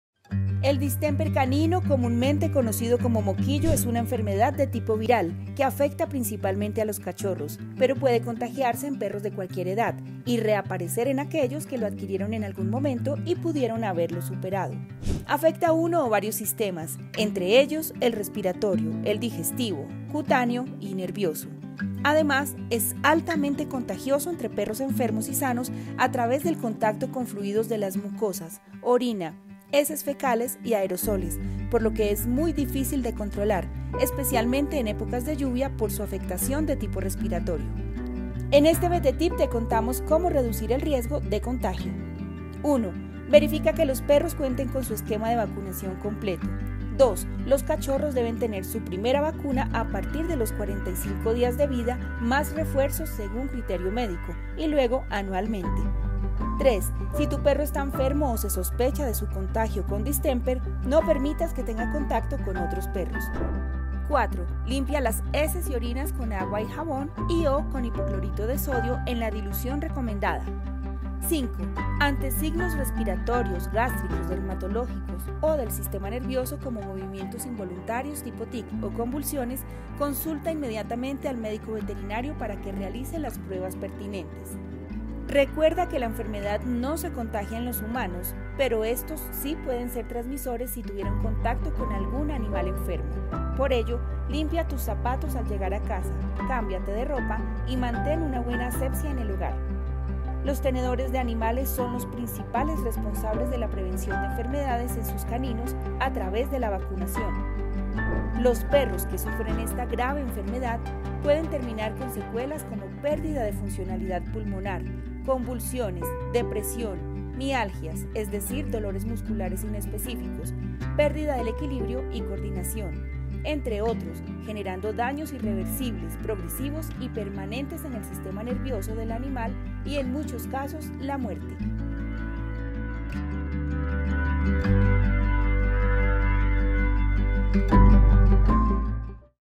Vetetip Prevención del distemper canino
En este Vetetip te contamos qué es el moquillo y cuáles son sus consecuencias. 5 pasos para cuidar a tu perro de esta enfermedad.